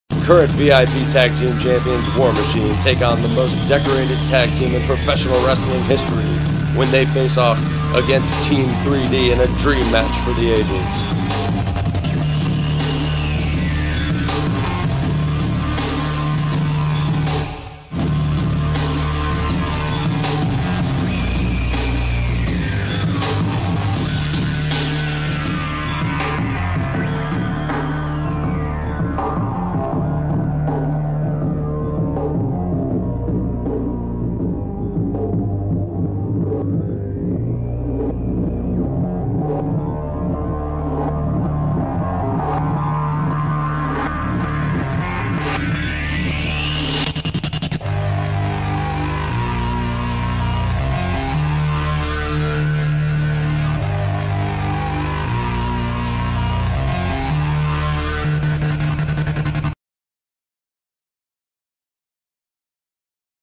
War Machine vs Team 3D
VIP Tag Team Champions WAR Machine take on the most decorated Tag Team in Wrestling History in Team 3D in A DREAM MATCH for the ages. prowrestlingdallas.com and ticketfly.com for tickets and more information!!